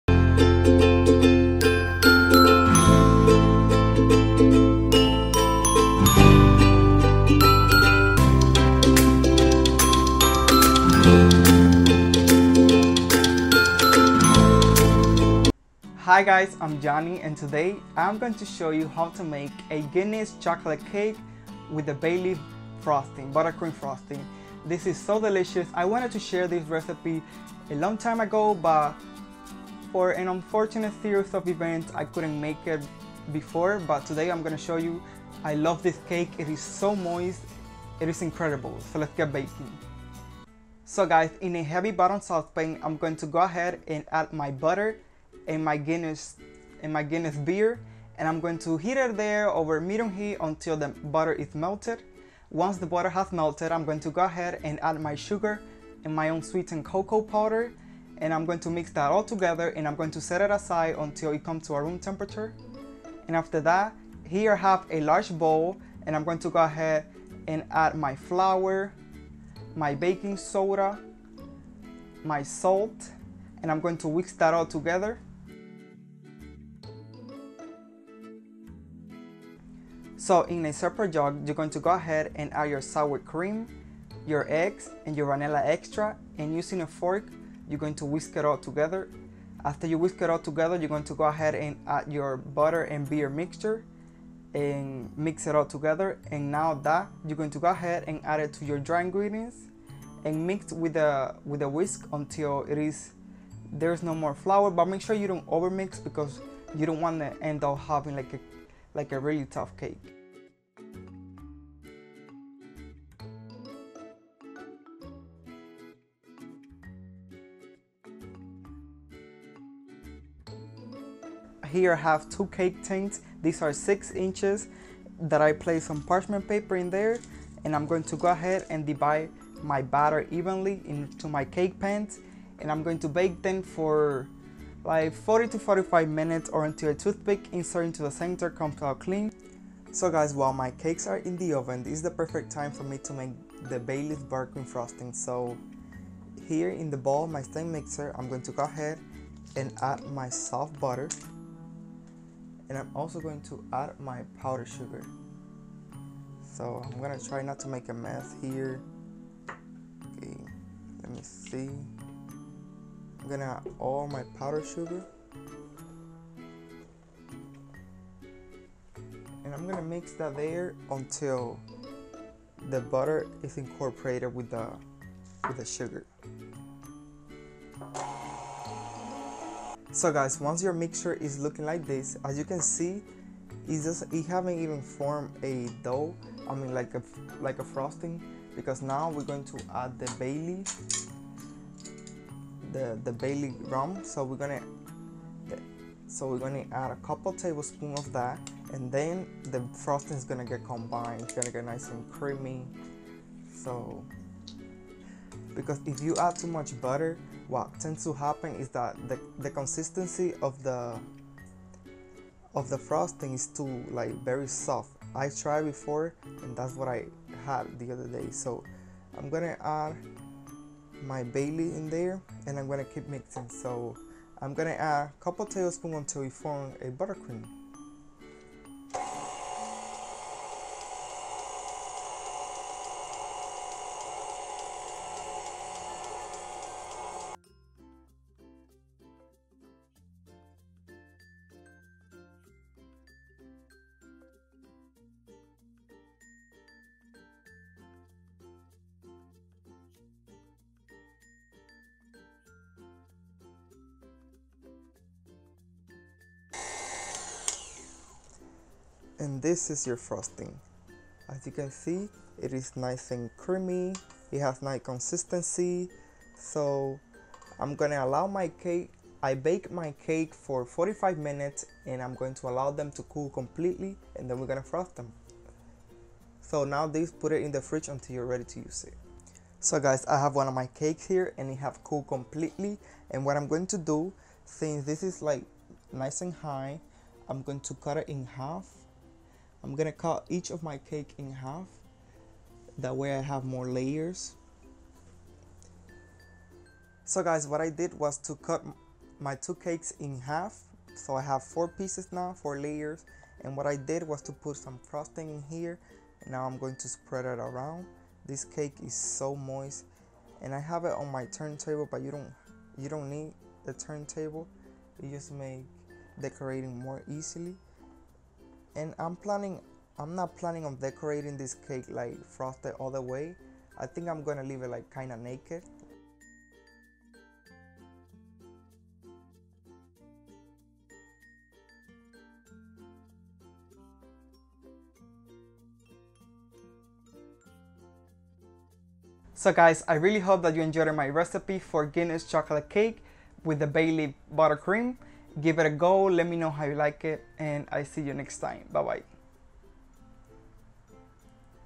Guinness Chocolate Cake With Bailey's Buttercream - Episode 201
This recipe is great for Father's Day :)

Ingredients:
For the cake
1-3/4 cup all purpose flour, sifted
3/4 cup unsweetened cocoa powder
2 cups of white sugar
3/4 cup butter
1 cup guinness extra stout
2/3 cup sour cream
2 large eggs
2 tsp vanilla extract
2 tsp baking soda
1/2 tsp salt

Bailey's buttercream:
1 (1 lb/16oz) box of powdered sugar
1 stick (1/2 cup) of butter
3-4 Tbsp bailey's

NOTE: you can bake them on 8 or 9 inch cake pans for 30-35 minutes. I used two 6 inches cake pans and took 45 minutes to baked.